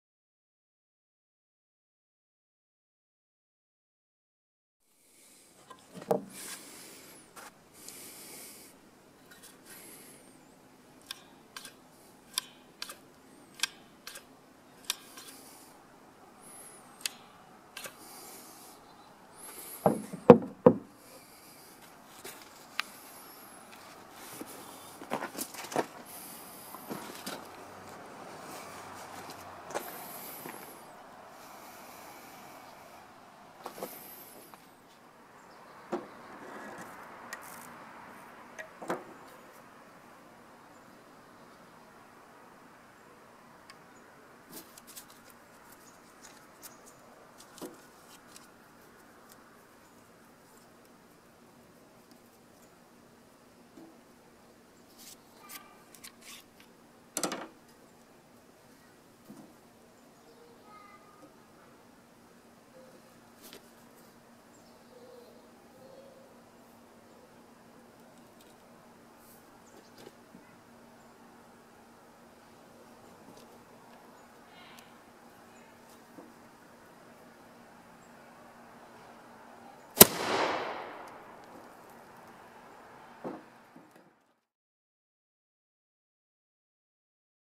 Serpentine Pistol
This is a 36 caliber Serpentine [pre-matchlock]  pistol  firing with 20 grains of Goex black powder.